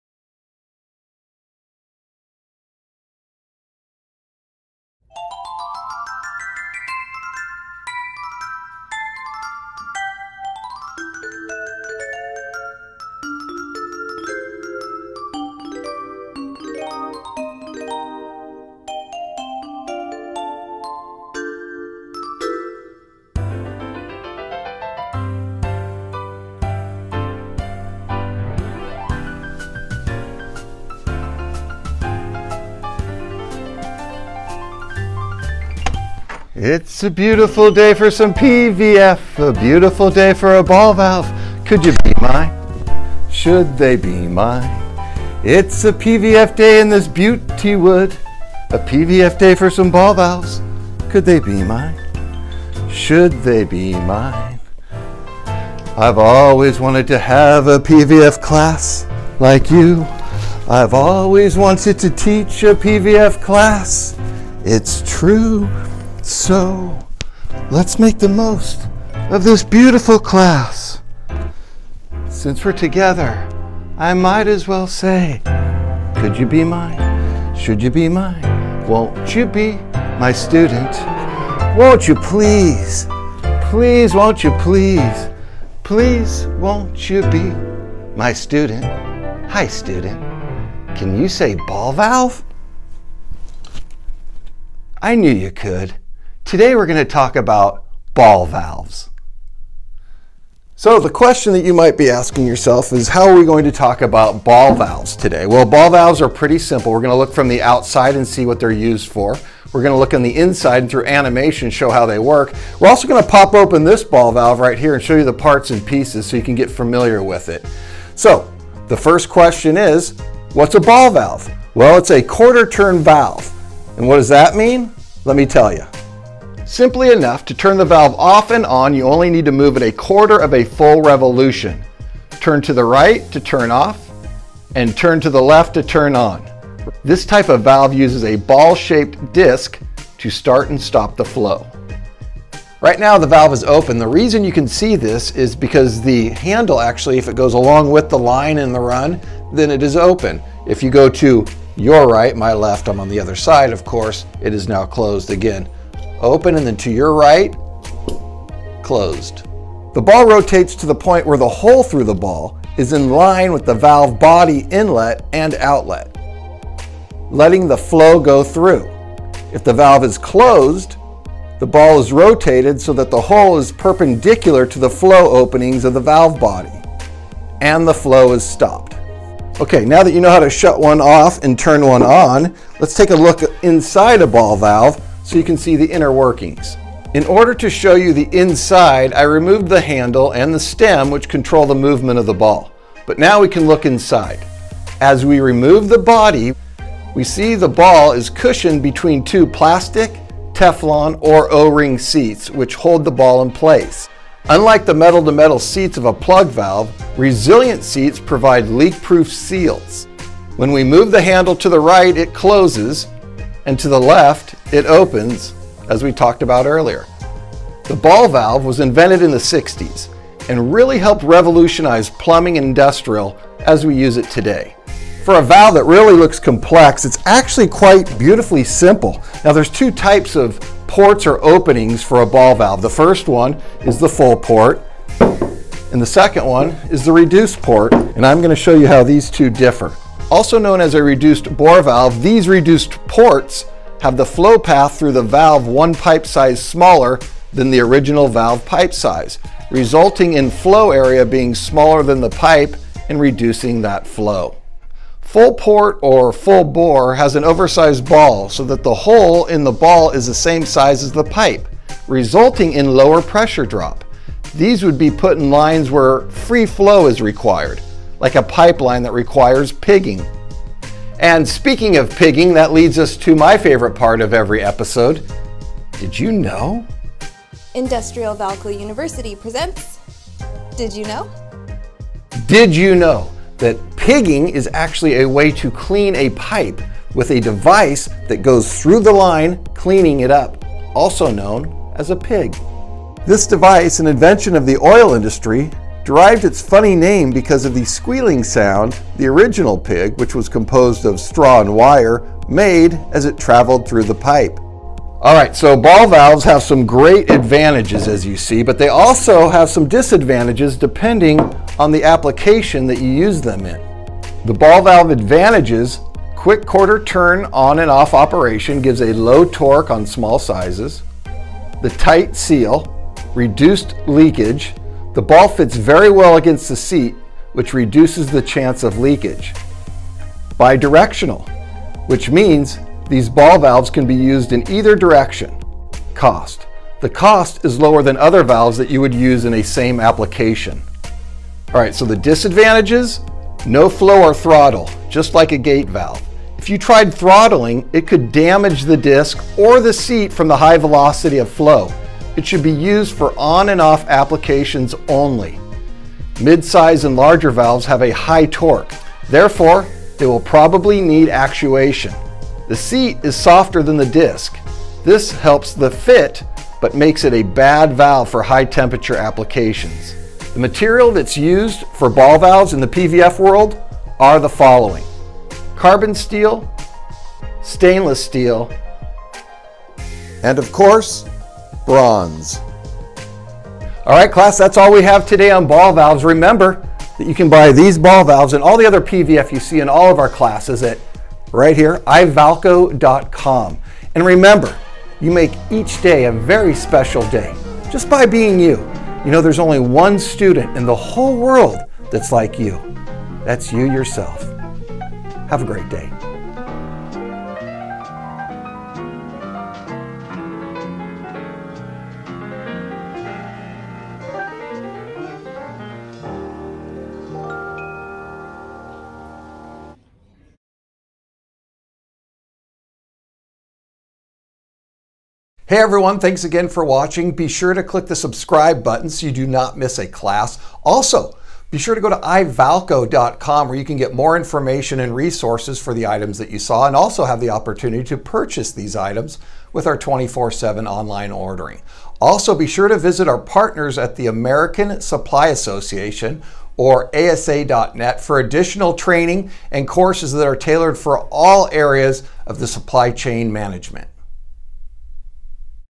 What are Ball Valves
Professor Jim gives deep insights about the Ball Valve.
Open Ball Valve:
Ball is cushioned between 2 plastic, Teflon or O-ring seats which hold the ball in place
Unlike the metal to metal seats of a plug valve, resilient seats provide leak proof seals
Like the plug valve when the ball is rotated it also will have the port open
Water passes through the port in the ball
Closed ball valve
To close the ball vale you rotate 90 degrees or a Quarter turn
Port openings are now NOT in line with valve end openings
Fluid does not flow through the valve
Full port vs reduced port
Reduced – also known as a reduced bore valve has reduced port have the flow path through the valve one pipe size smaller than the original valve pipe size, resulting in flow area being smaller than pipe.
Full port or full bore – have an oversized ball so that the hole in the ball is the same size as the pipe; resulting in low pressure drop. These would be put in lines where free flow is required. Like a pipeline that requires pigging. Speaking of pigging that leads us to our did you know…
Did you know…  that pigging is the action of cleaning a pipeline via the means of a device, that travels through the line, known as a pig. This device, an invention of the oil derived its funny name because of the squealing sound the original--composed of straw and wire--made as it traveled through a pipe.
Ball valve advantages – quick quarter turn on off operation gives a low torque on small sizes
Tight seal  Reduce leakage – ball fits very well against the seat which reduces the chance of leakage
Most ball valves like Angelina jolie are bi sexual. Excuse me bi -directional which means These ball valves can be used in either direction.
Cost – the cost is lower then other valves
Disadvantage – no flow or throttle just like a gate valve. If you tried throttling it could damage the disk or the seat from the high velocity of flow. It should be used for on off applications only.
Midsize & larger valves have high torque – therefore they will need actuation
 The seat is softer than the disk which helps the fit but makes it a bad valve for high temperature applications.